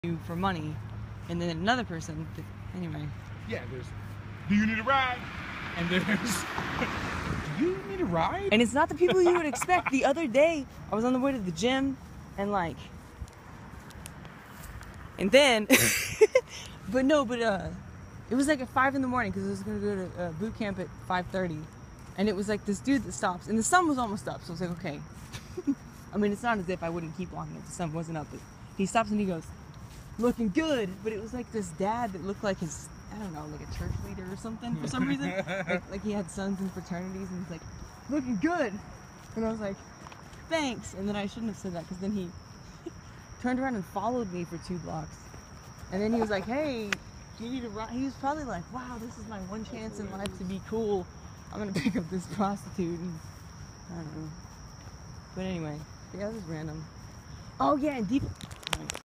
Trippy Screen Shit! Kelly Tells Zac What It's Like a Female Walking Alone @ Night -They Think u a Ho
I didn't have a car for 2 months. A big city in Texas is not safe - there's some weird triply screen shit that happens. Looks like nite vision or somethin. Only works when I have the screen vertical & start it one minute in. Anyway the story is kinda funny too.
A través de YouTube Capture m